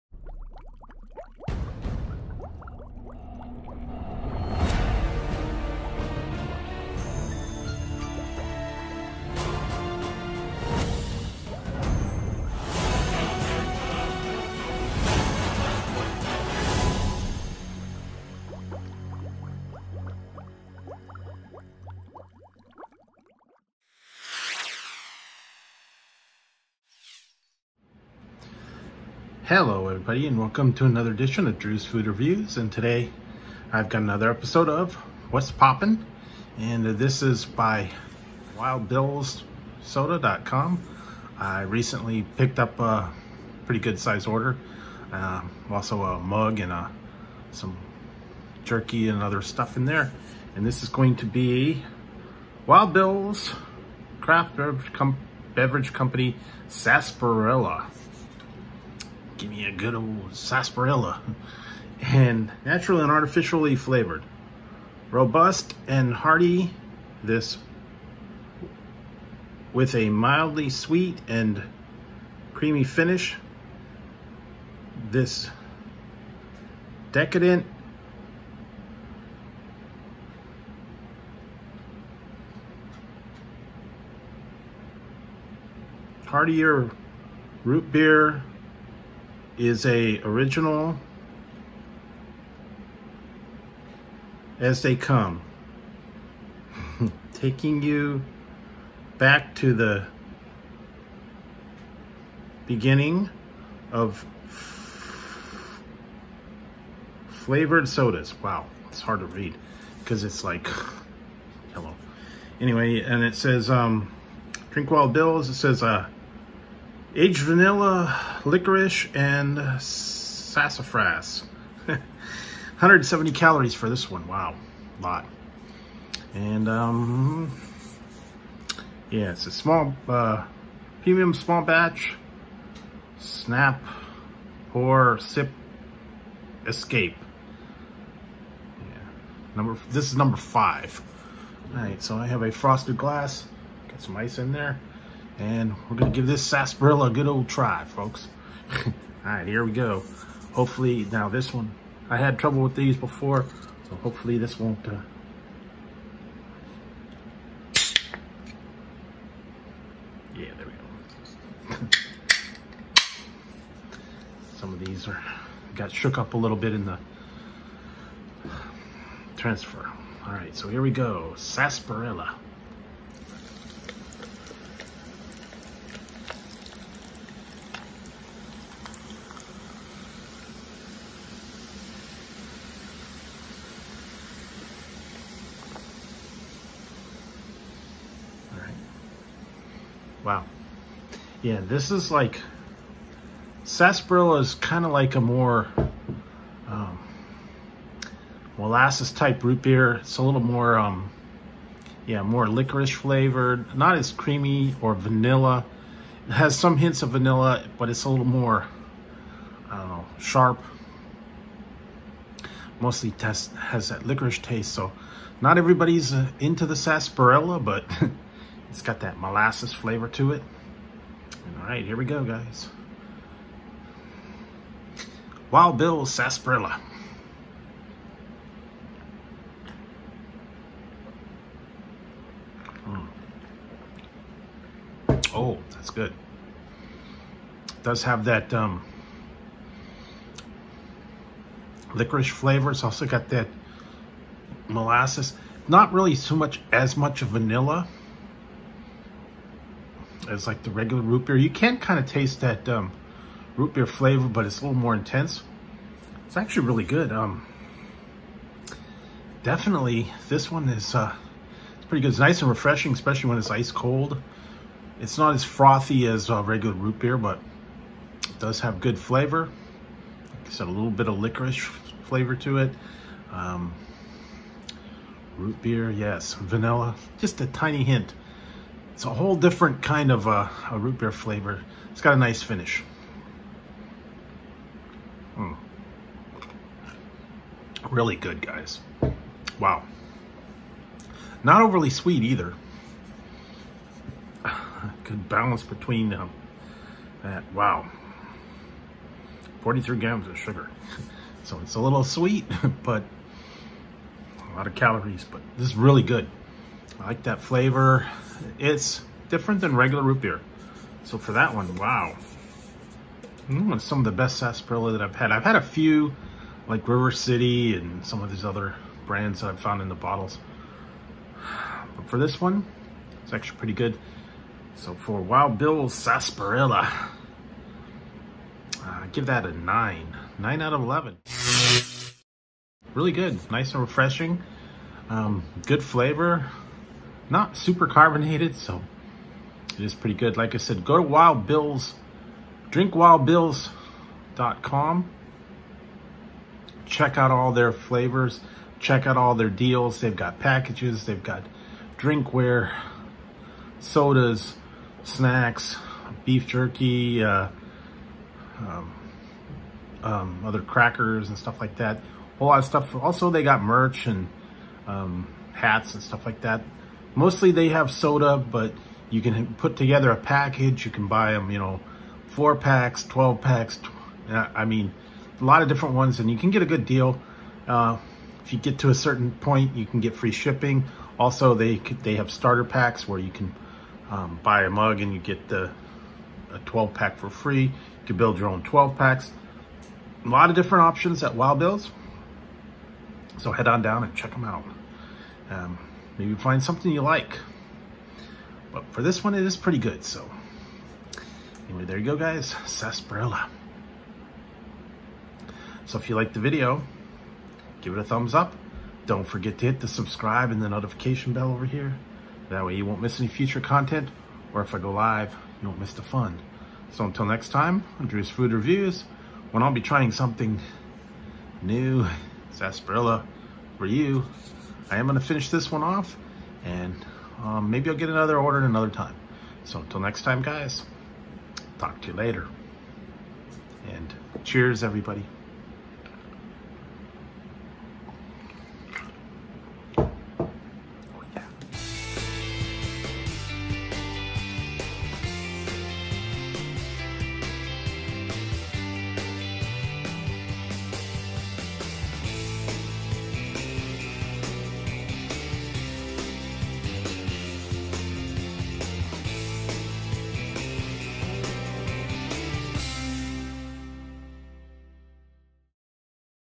What’s Poppin : Wild Bills Sarsaparilla
Today on Whats Poppin I’m trying wild bills sarsaparilla soda you can check out wild bills soda and items here